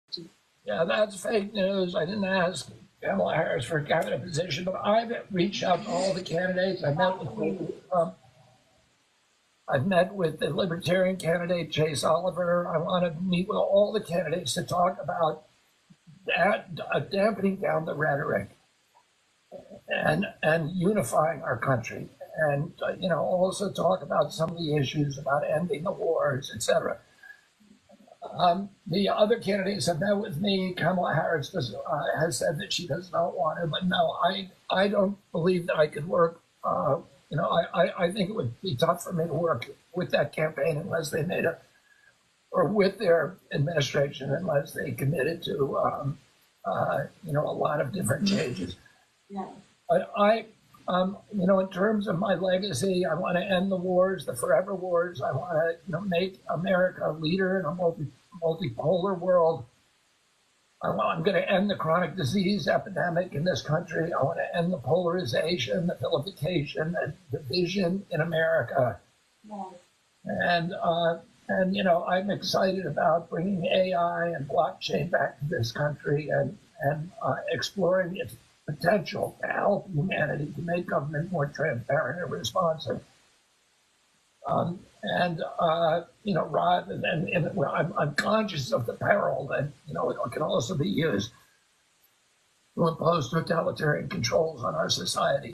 RFK Jr. Did Not Ask For Position In Kamala Admin
He does want all of the candidates to tamp down the rhetoroc.